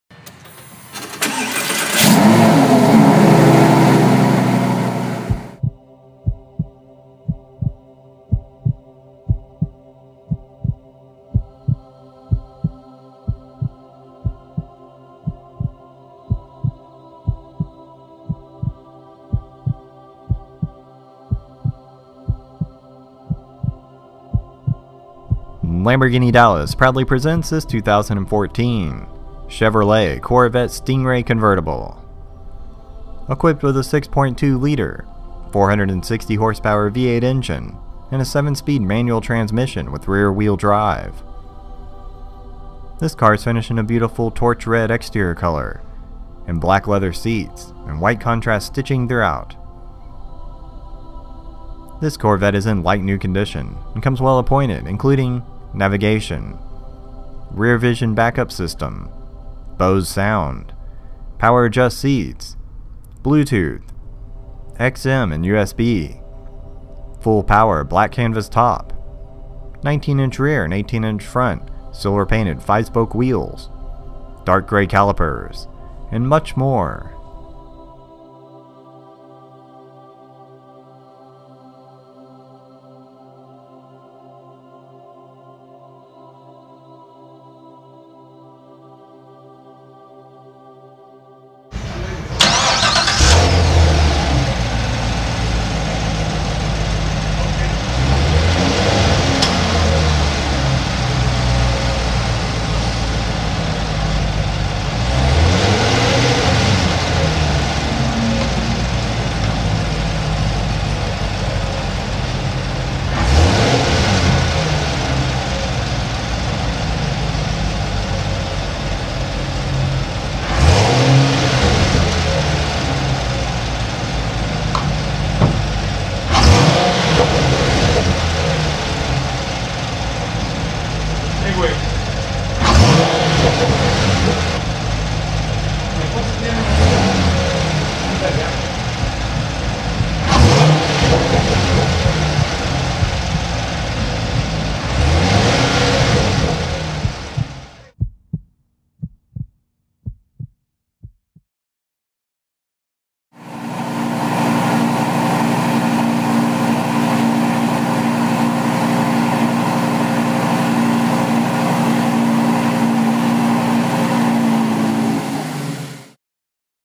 2014 Chevrolet Corvette Stingray Convertible Torch Red LC435
This is a smoke free Carfax certified 2014 Chevrolet Corvette Stingray convertible equipped with a 6.2L 460HP V8 engine and a 7-speed manual transmission and RWD. This car is finished in a beautiful Torch Red exterior color and Black leather seats and white contrast stitching.

This corvette is in "like new" condition and comes well appointed including:
+ Navigation
+ Rear vision back up system
+ Bose sound
+ Power adjust seats
+ Bluetooth
+ XM, USB
+ Full power black canvas top
+ 19" rear / 18" front silver painted 5-spoke wheels
+ Dark Gray calipers